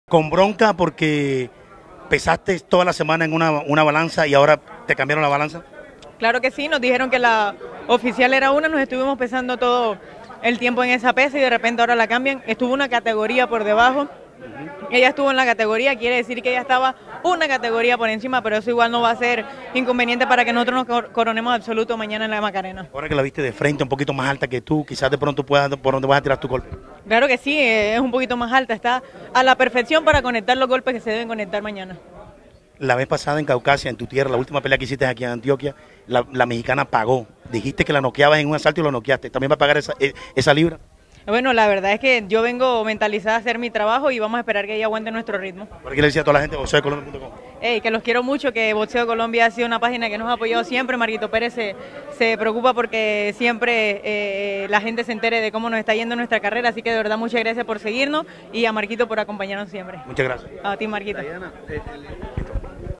Dayana Cordero vs Mayerlin Rivas pesaje weigh- in
Dayana Cordero vs Mayerlin Rivas pesaje weigh in wba pesaje Medellin Colombia Marco Perez